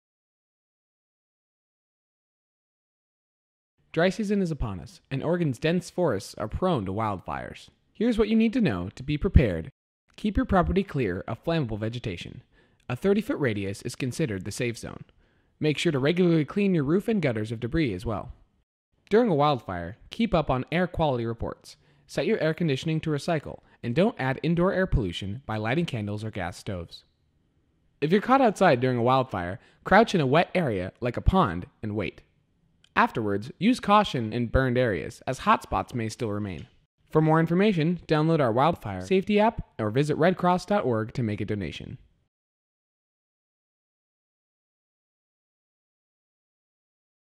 Red Cross Wildfire Safety
How to prepare your home to avoid being affected by wildfire; Red Cross Wildfire app